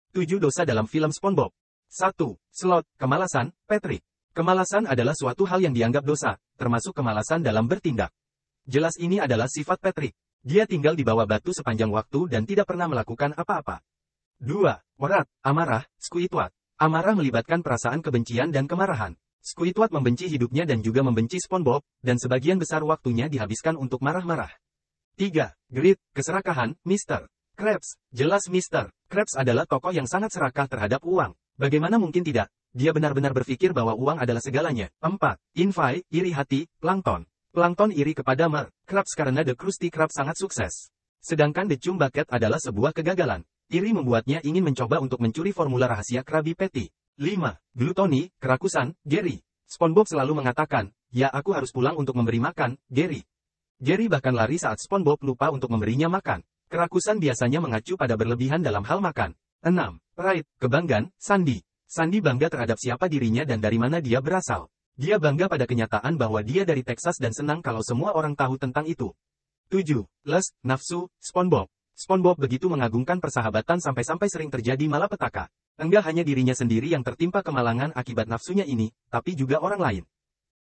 7 dosa dalam film SpongeBob SquarePants
Mungkin banyak yang tidak sadar dengan karakter -karakter di film kartun Spongebob yang menggambarkan dosa- dosa besar manusia.

Diketahui film kartun Spongebob ini merupakan serial kartun yang banyak di tonton oleh akan-akan.

Lanatas apa saja dosa-dosa besar manusia yang digambarkan di film serial Spongebob.

seven deadly sins (tujuh dosa mematikan)